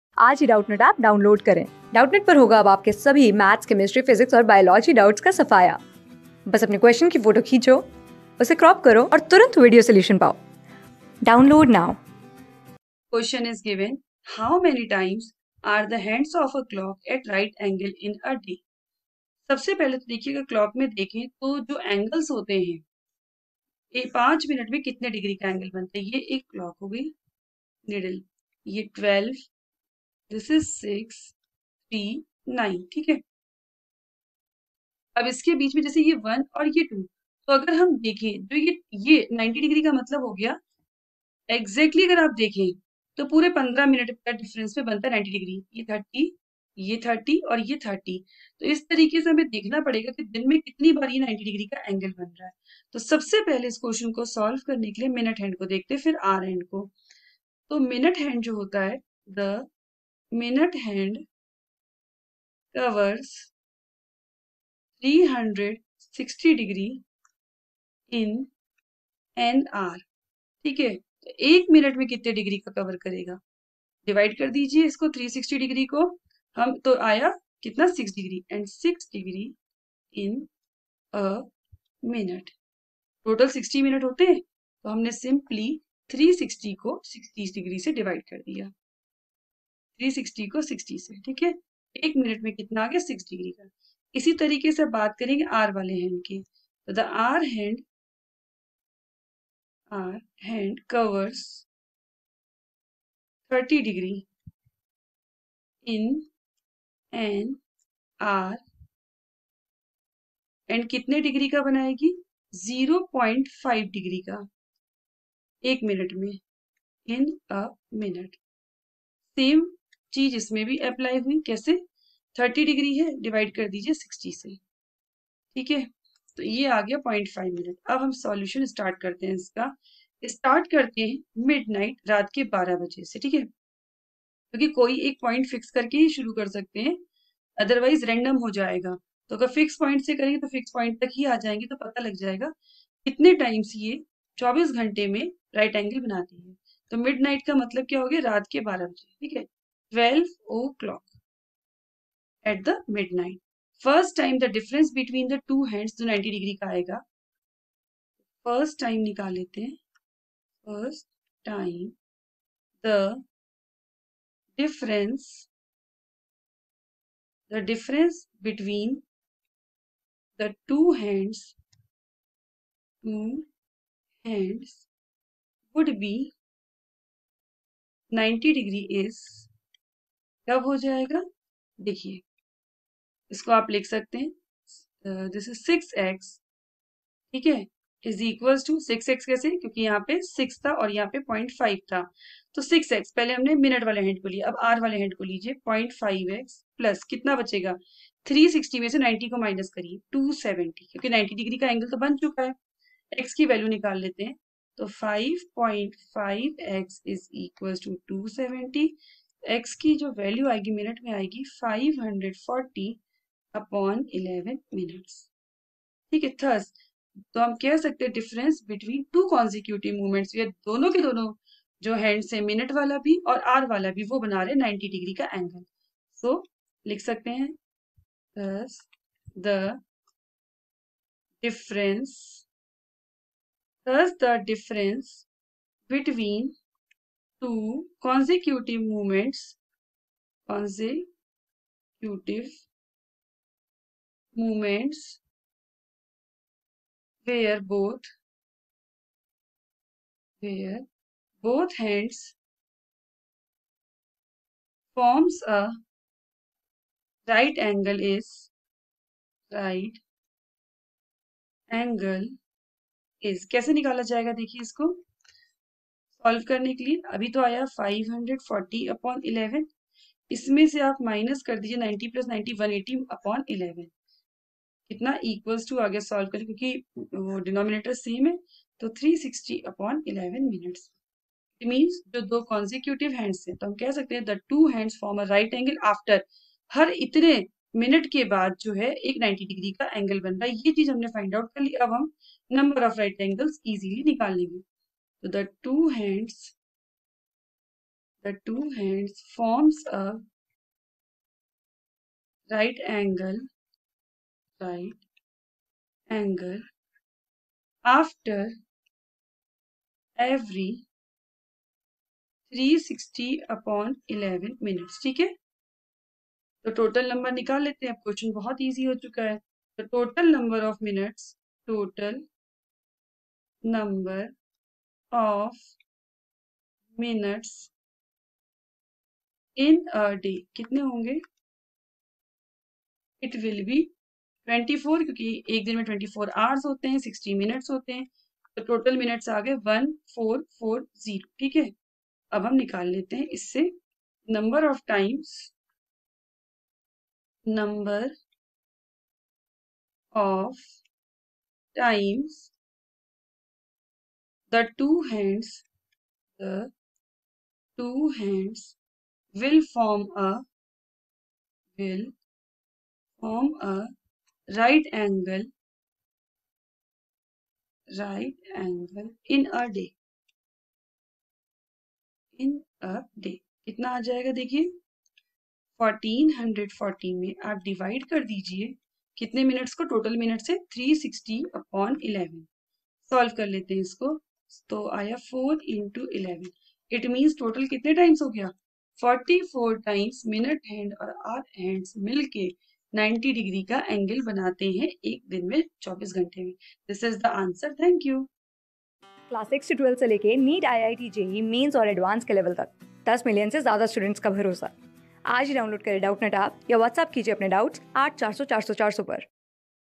How many\r\ntimes are the hands of a clock at right angle in a day?\n(a) 22 (b) 24 (c) 44 (d) 48...
Class: 14
Subject: MATHS
Chapter: CLOCKS
Board:CBSE

👉 Have Any Query? Ask Us.